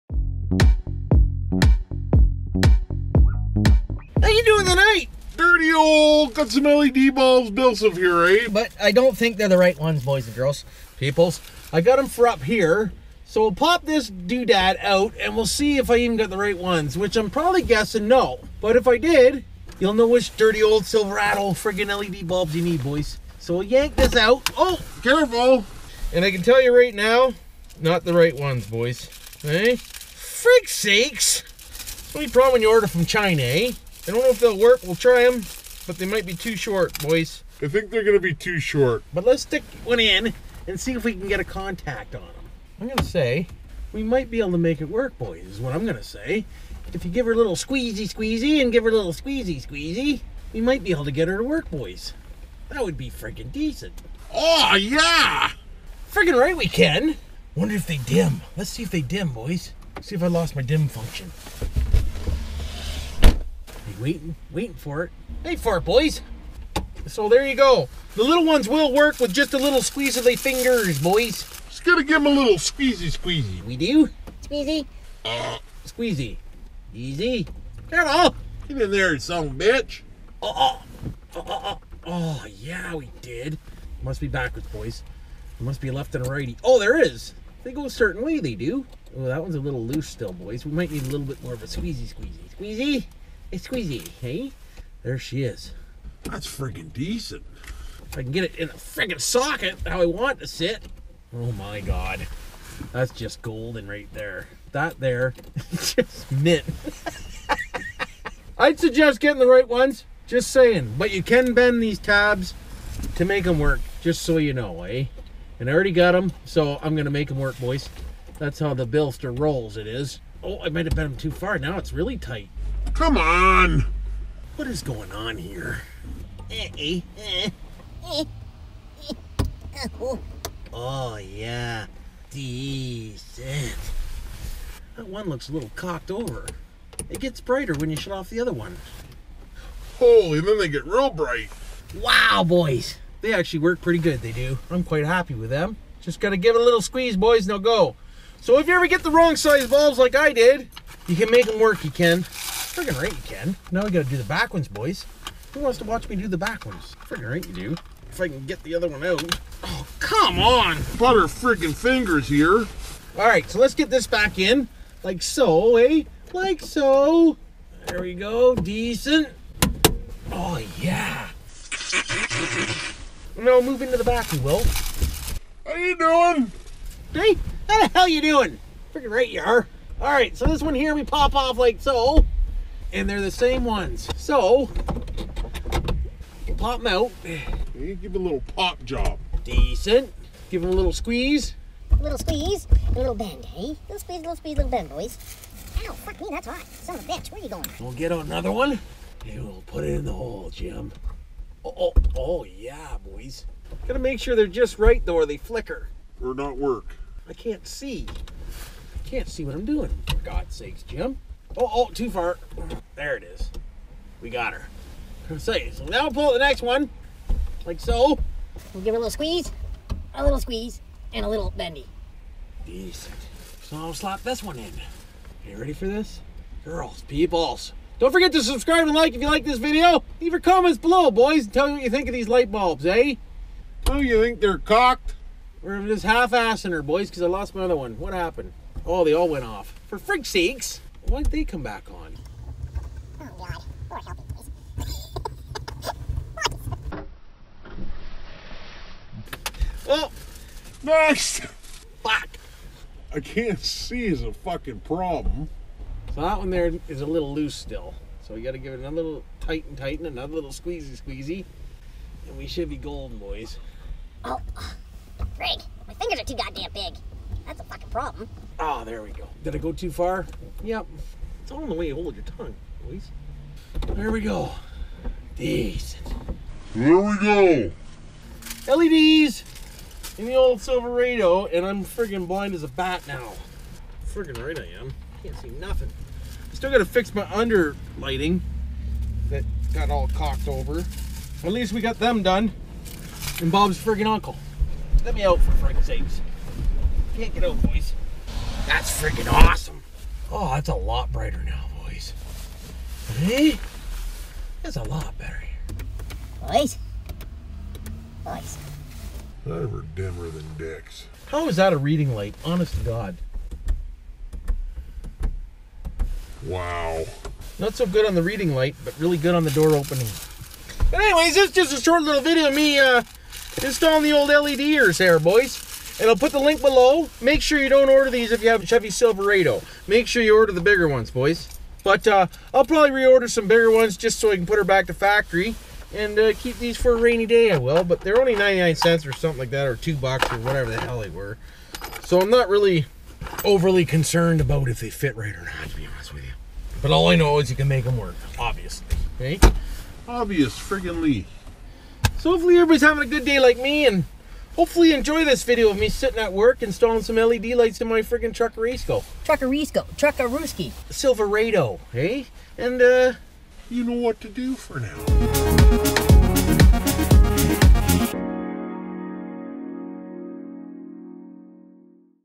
How To Install The WRONG LED Bulbs In A Silverado
Just installing some less that are not for my truck . They work Mint!

Here they are ppl's

BillSiff (GoGlass)
1289 Confederation St,
Unit 3
Sarnia Ont.
Canada
N7S 4M7